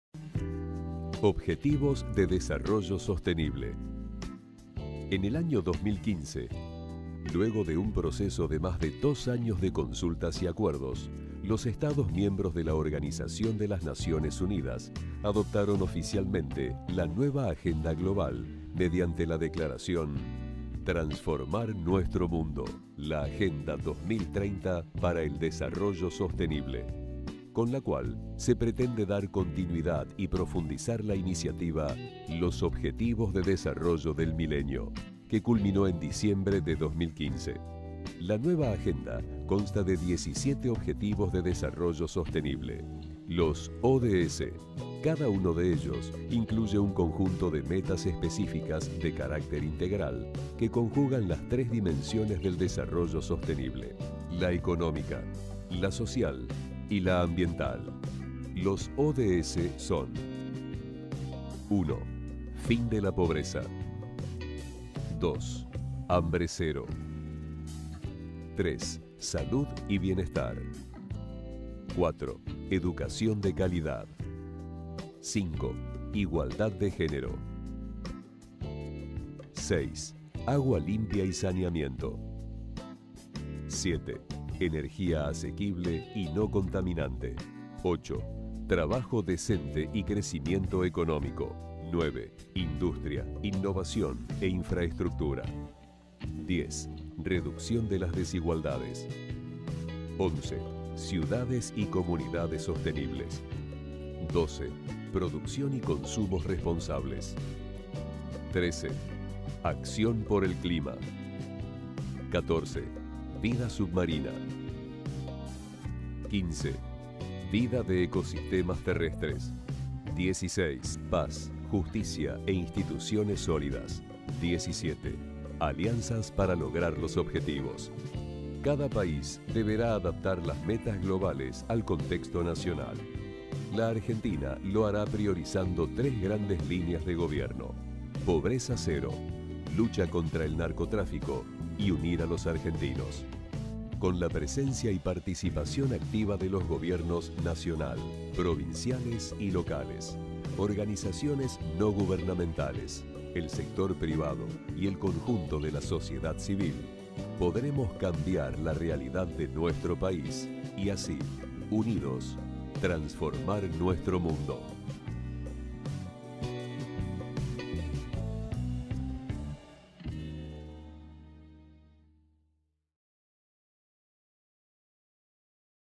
Presentación de los Objetivos de Desarrollo Sostenible ODS.
-Fuente: Consejo Nacional de Políticas Sociales.

Los Objetivos de Desarrollo Sostenible (ODS) fueron aprobados por los dirigentes mundiales para poner fin a la pobreza en todas sus formas, reducir la desigualdad y luchar contra el cambio climático. El programa se extiende desde el año 2015 al 2030 y plantea 17 objetivos cuyas metas varían dependiendo de la realidad de cada país. Fueron acordados por los 190 países que constituyen la Asamblea General de Naciones Unidas.